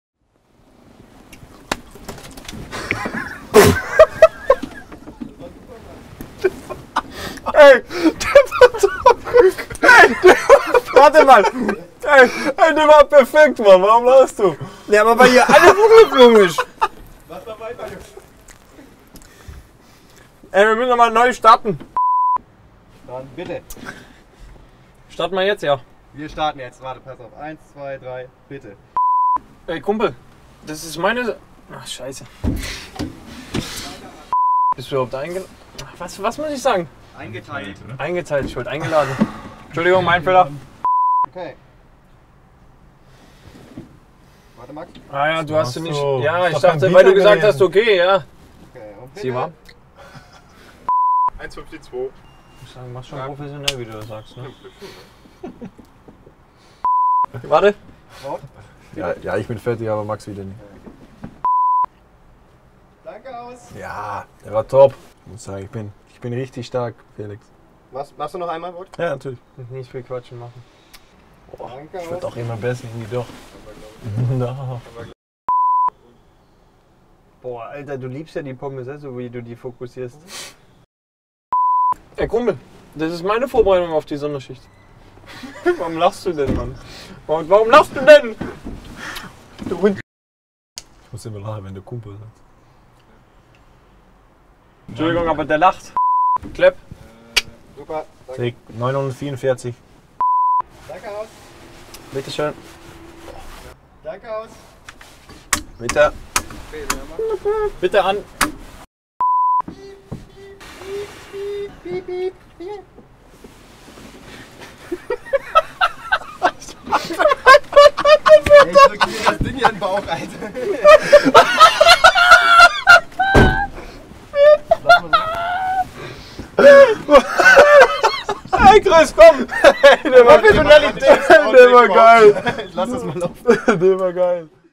"Warum lachst du denn jetzt 🙈😂?" | Sonderschicht Outtakes mit Wout Weghorst & Max Arnold
Wout Weghorst und Maximilian Arnold sind richtige Spaßvögel. Das stellten sie auch beim Dreh der Sonderschicht wieder unter Beweis. Es klappte nicht alles auf Anhieb, umso besser wurden die Outtakes. Viel Spaß damit!